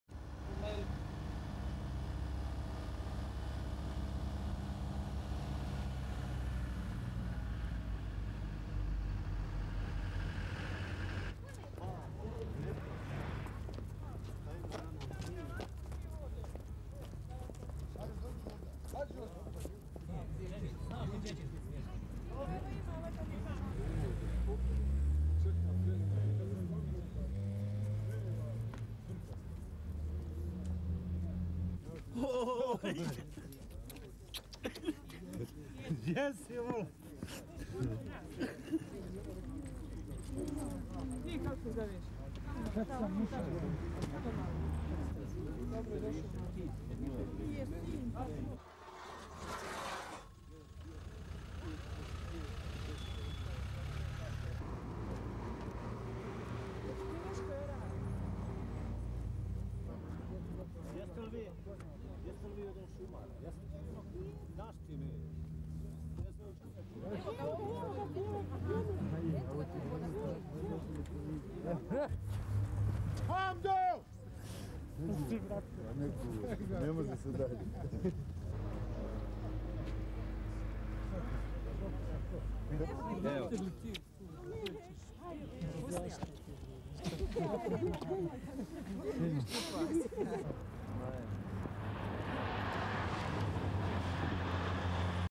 Bosnia - Exchange Of Prisoners
(11 Oct 1994) An exchange of Bosnian and Serb prisoners took place on Monday
(10/10) across the Brotherhood-Unity bridge in Sarajevo.

night shots of red cross trucks
families carrying belongings
relatives waiting
gvs of families reunited
soldier making way for red cross vehicle
u.n. soldier carrying suitcase
man speaking to children
more hugging
people walking off

1.35